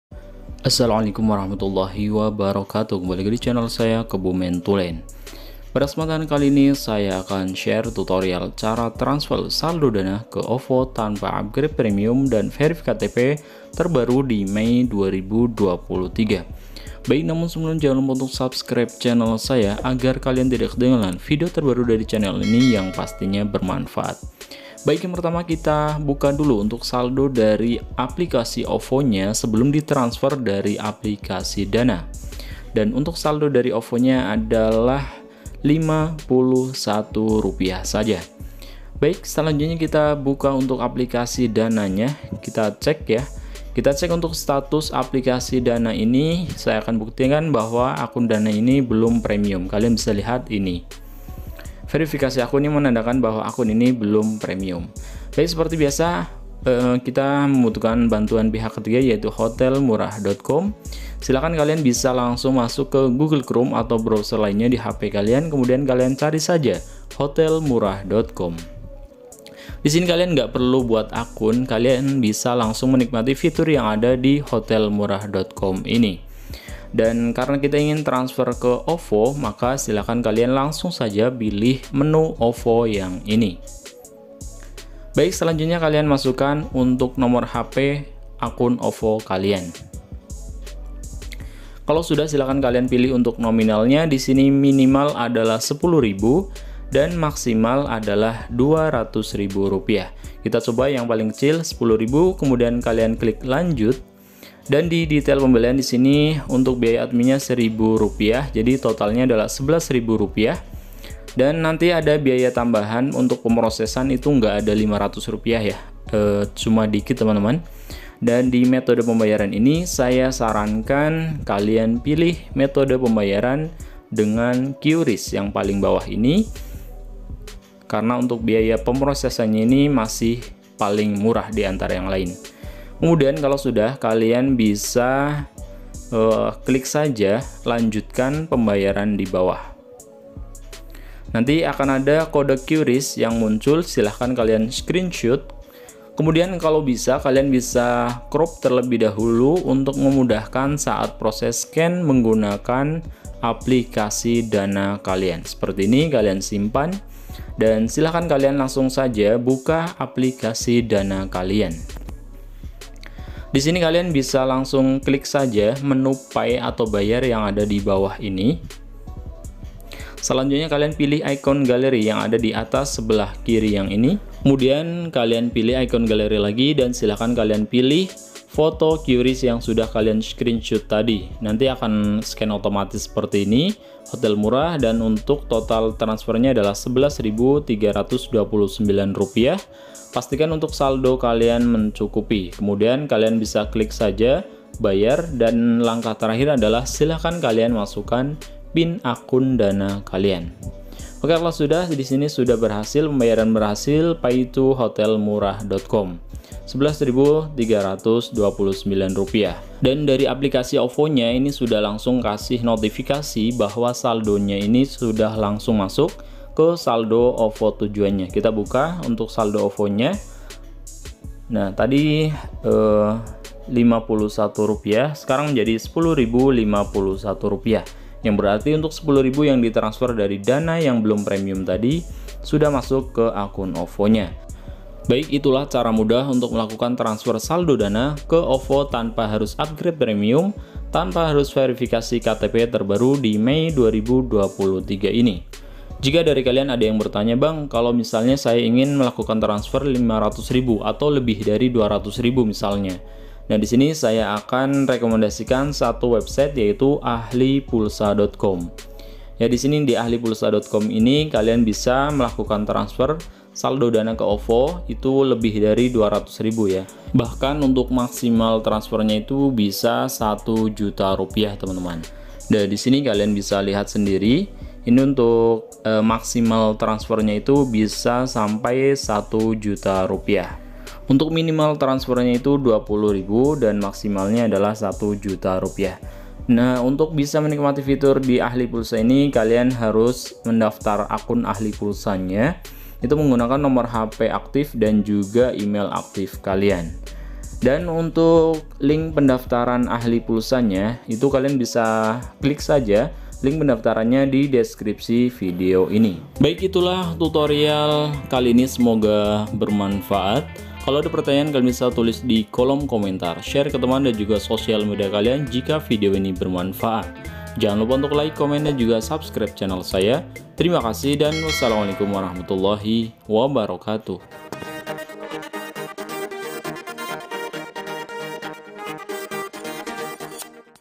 Cara transfer dana yang belum upgrade premium ke ovo | transfer dana ke ovo tanpa verifikasi ktp
Hai Sobat Tulen,
Kali ini saya buatkan tutorial update terbaru mei 2023 Tutorial Transfer Saldo Dana Yang Belum Upgrade Premium Ke OVO.

Dan info buat teman-teman, saya sarankam jangan melakukan transaksi di jam 22.00 wib s.d jam 05.00 wib, takutnya saldo masuk lama atau nggantung karena serever sedang maintenance dll.